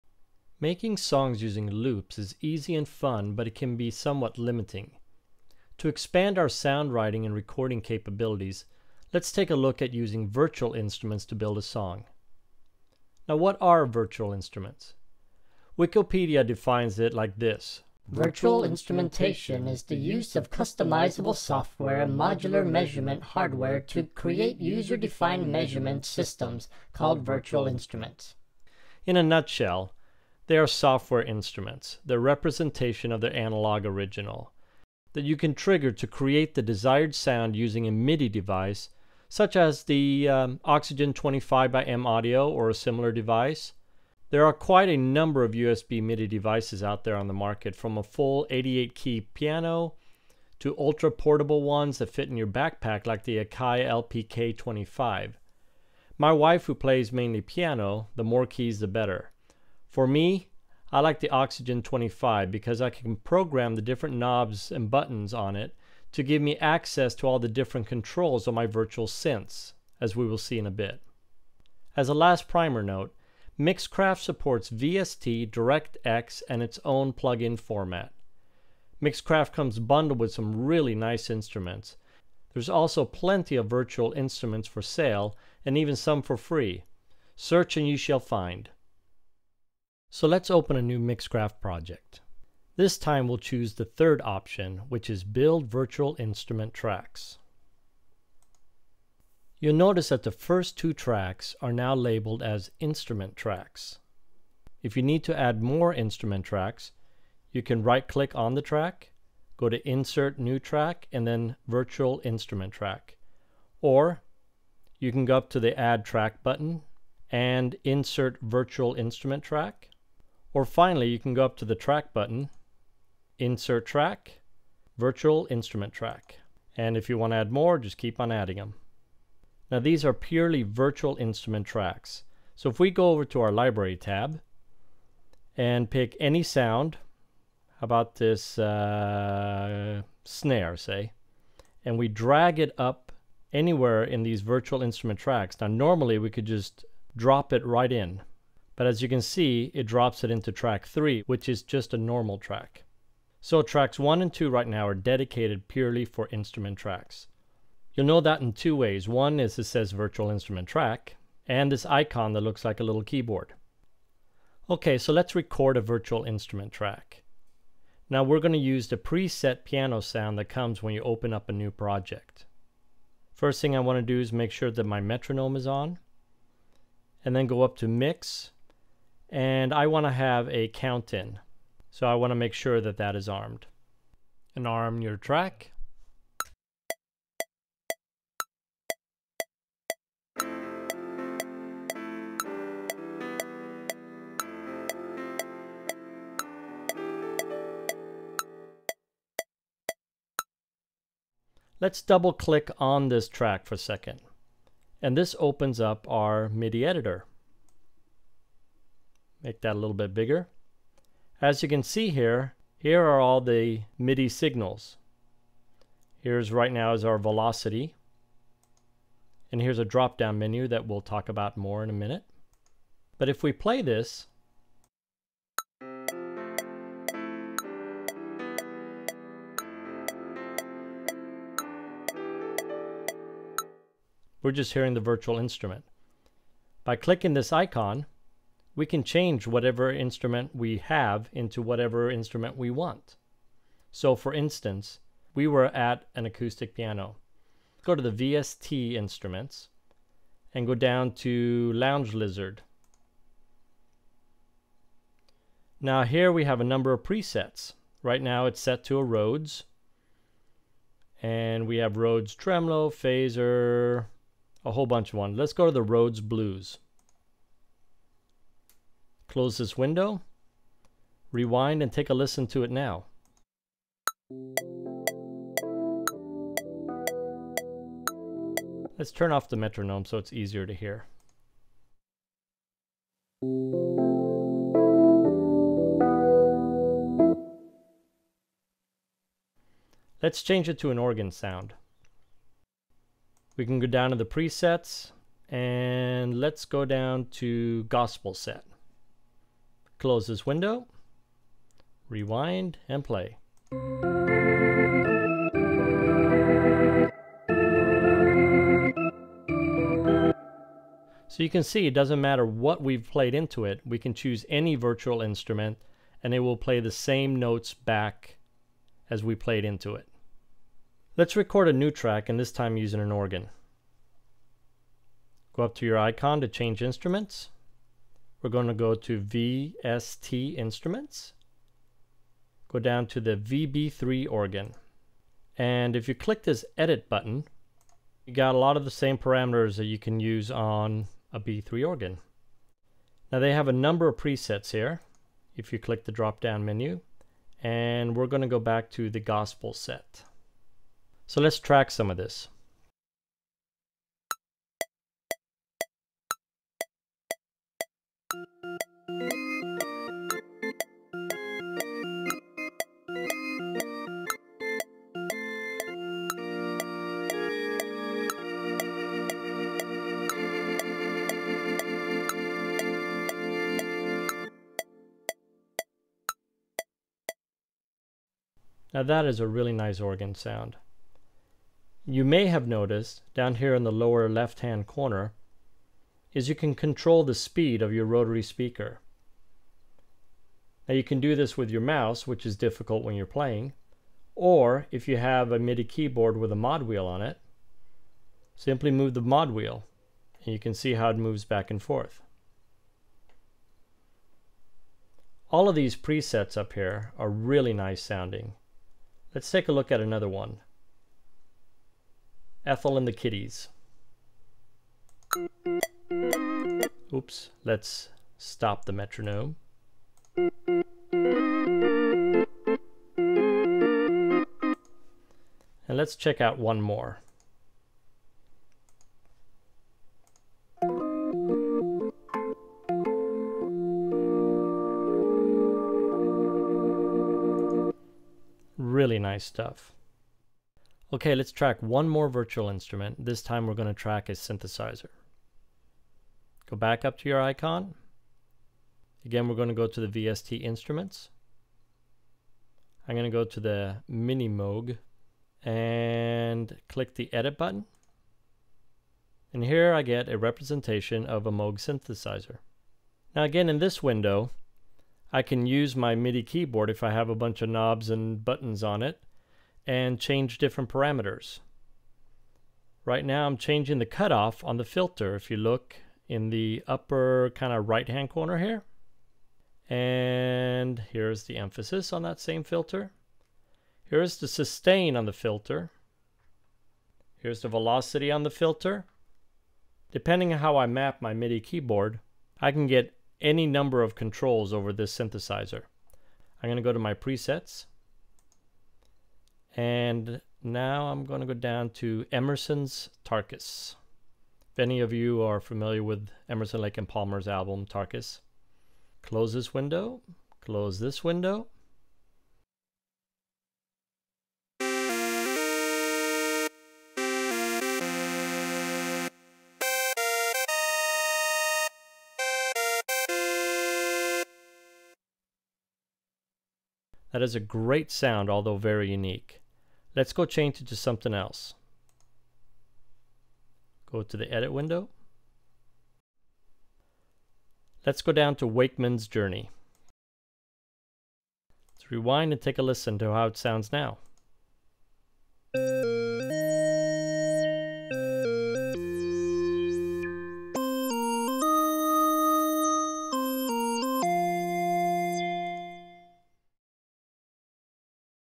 Virtual Instruments - Recording - Mixcraft 5
This video addresses how to record virtual instruments in Mixcraft 5.